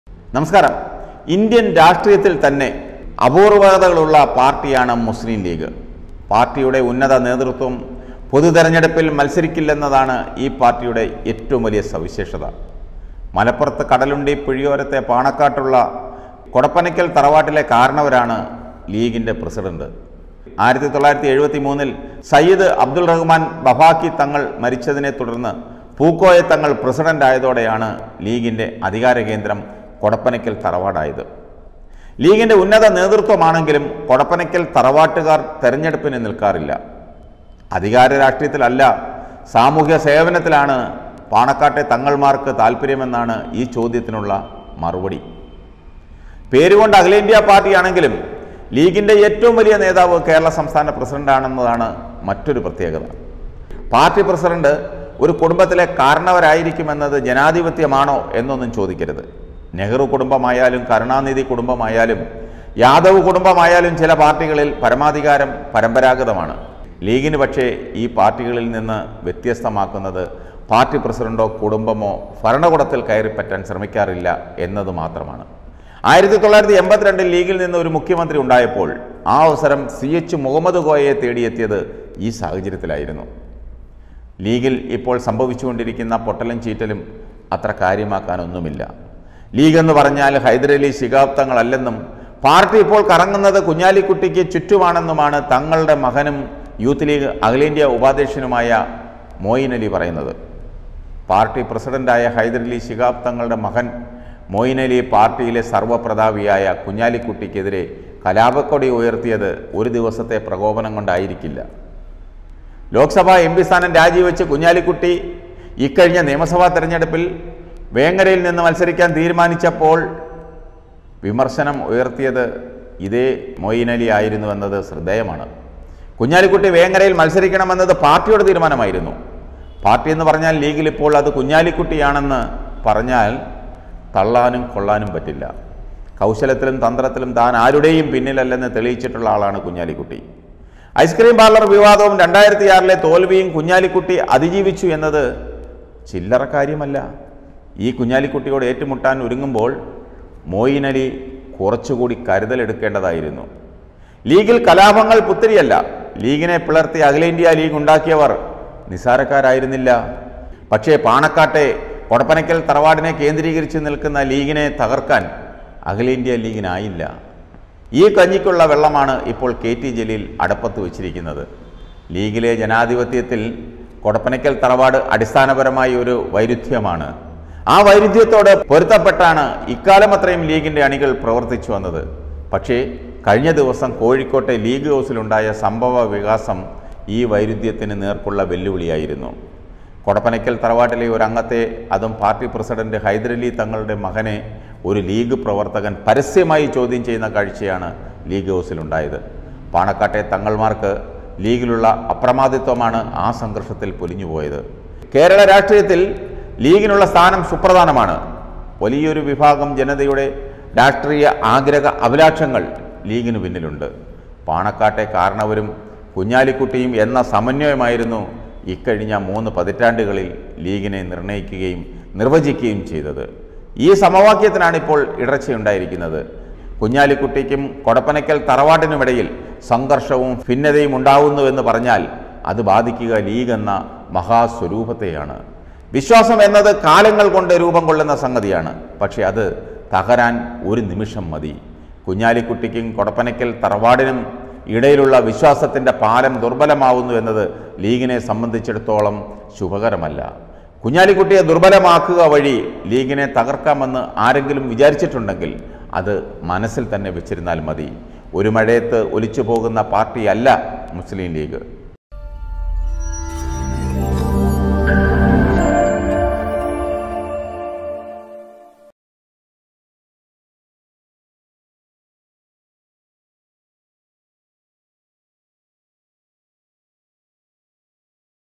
ലീഗിനെയും കുഞ്ഞാലിക്കുട്ടിയെയും തകർക്കാൻ നീക്കം : മനസ്സിൽ വച്ചാൽ മതി | News Updates
ലീഗിനെയും കുഞ്ഞാലിക്കുട്ടിയെയും തകർക്കാൻ നീക്കം : മനസ്സിൽ വച്ചാൽ മതി

Tag-Generator-Youtube-malayalam news-global